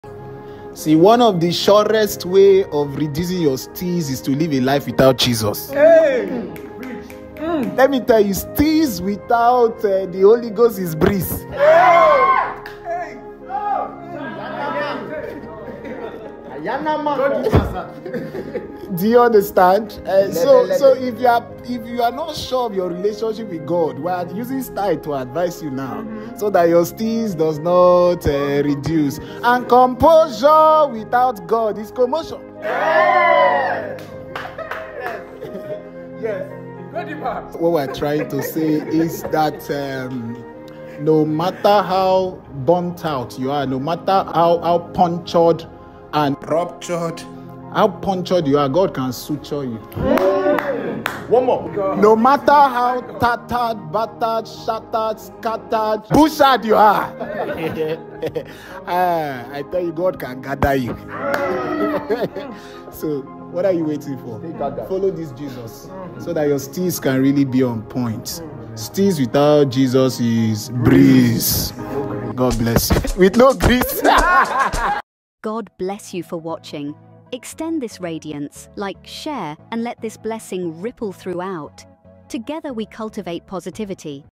Did MOSES BLISS and The SPOTLITE NATION Go Secular😳 Moses Bliss Responds Critics🔥
A valid message from Moses Bliss and the spotlite nation you need to listen to this….